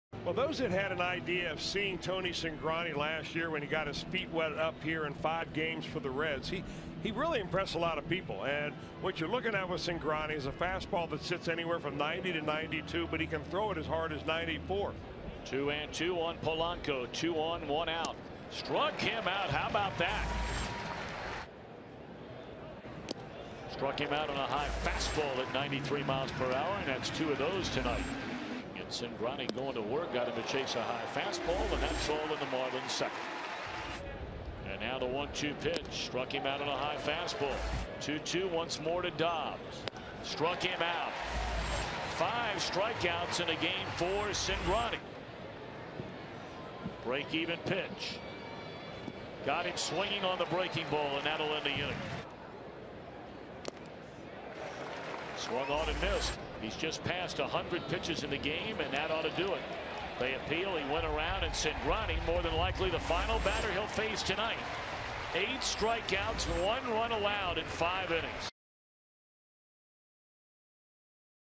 MIA@CIN: Cingrani impressive in first MLB start
4/18/13: In his first career MLB start, Tony Cingrani throws five innings of one-run ball while striking out eight Marlins batters